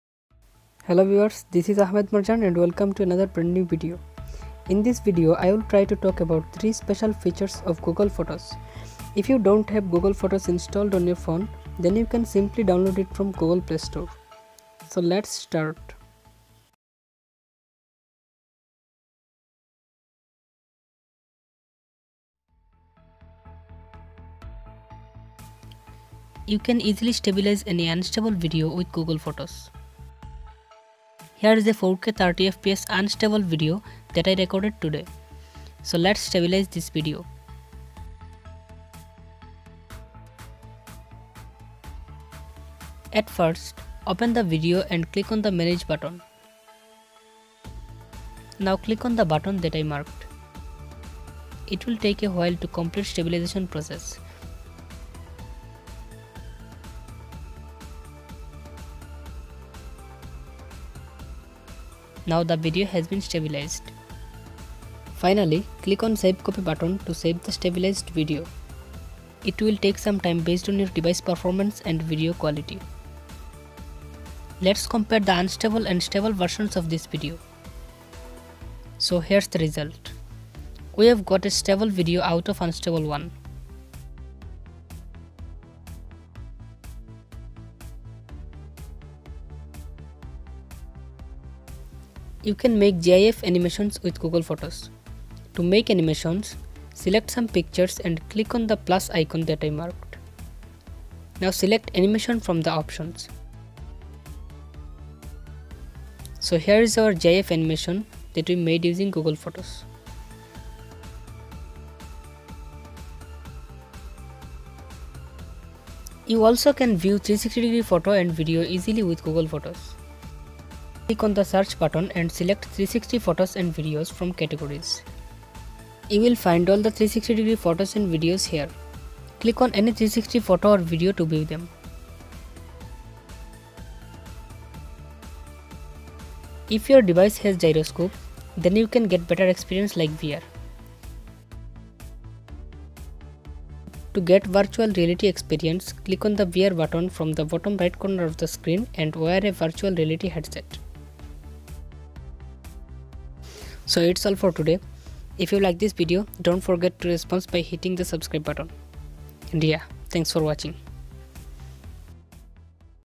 3 Special Features of Google Photos | Stabilise Unstable Videos | 360 Photo and Video | Animation.
Hello guys. Welcome to another video. In this video, I tried to talk about three special features of the app Google Photos. Definitely Google Photos is one of the best image viewers. So, watch this video till the end.

Intro:  0:00
Feature 1:  0:22
Feature 2:  01:28
Feature 3:  01:51
Outro:  02:34